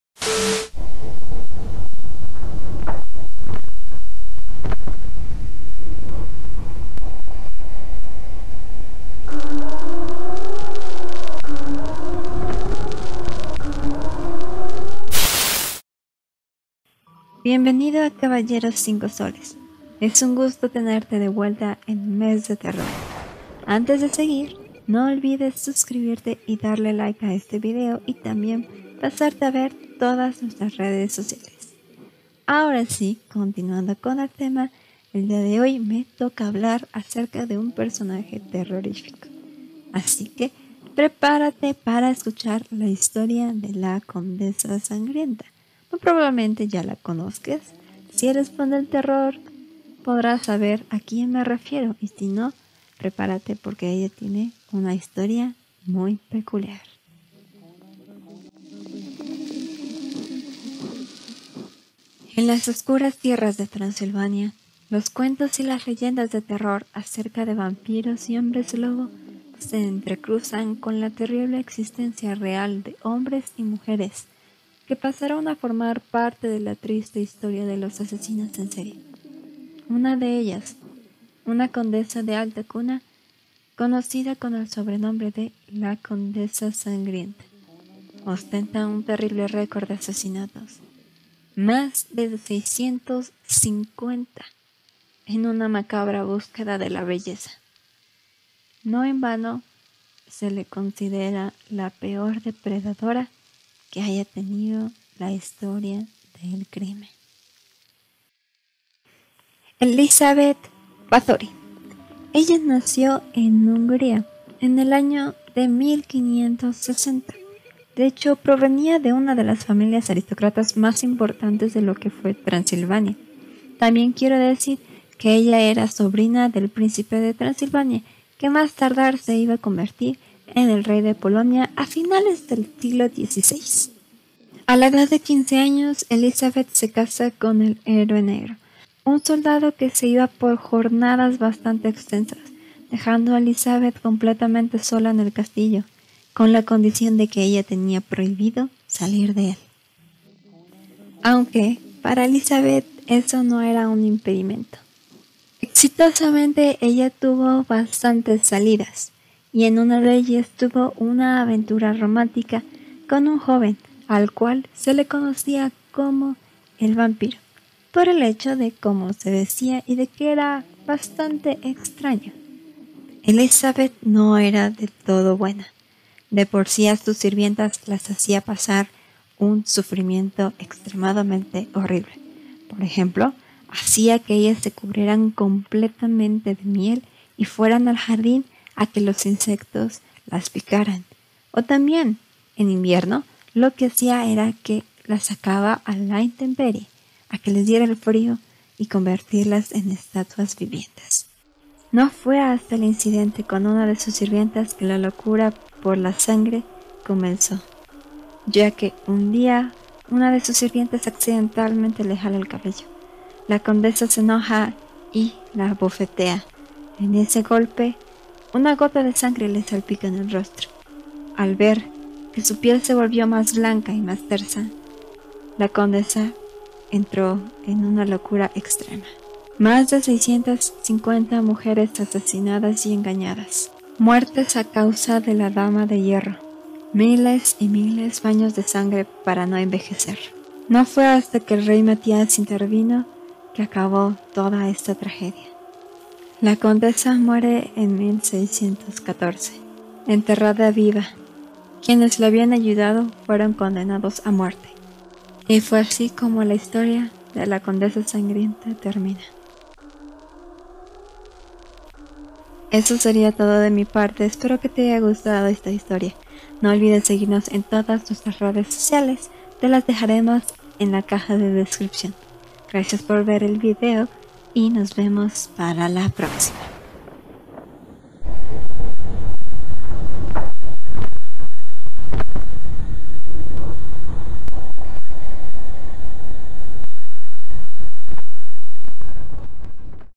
LA CONDESA SANGRIENTA / C5S Personajes
Esta noche hablamos sobre la mujer que aterrorizó Hungría en el siglo XVI: Elizabeth Báthory... La Condesa Sangrienta

Follow para apoyar a caballeros5soles